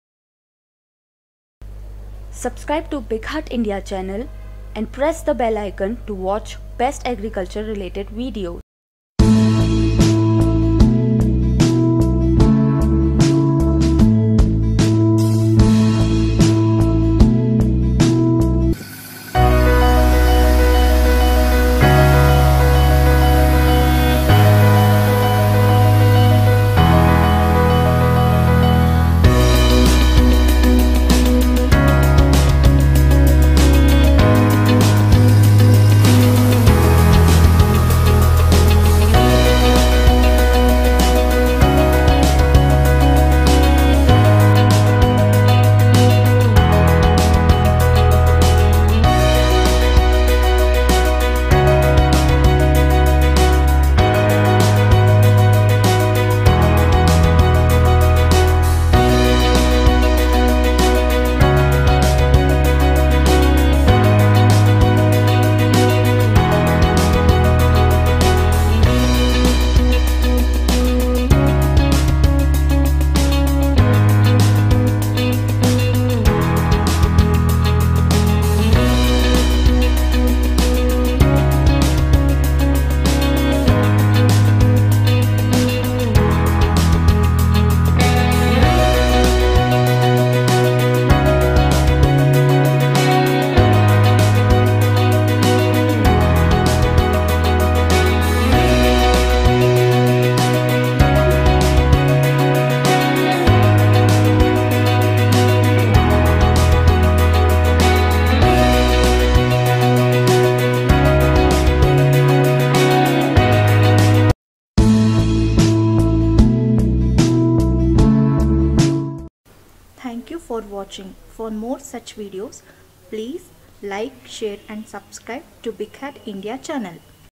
Air Blast Sprayer (Aerotic Turbo) Demonstration.
Proven Chemical and labor saving
 Proven Dipping Results
 Uniform coverage giving best crop protection
 Doorstep service and Installation by the trained service engineer
 1-year warranty with free service*
 5-mode controller and two way nozzles provides accurate delivery of chemicals
 Agitators provided for constant chemical concentration while spraying
 Diaphragm pump to eliminate pulsation in sprayer
 Airotec turbo 600 is also available in double fan variants.

Video Courtesy: Mitra.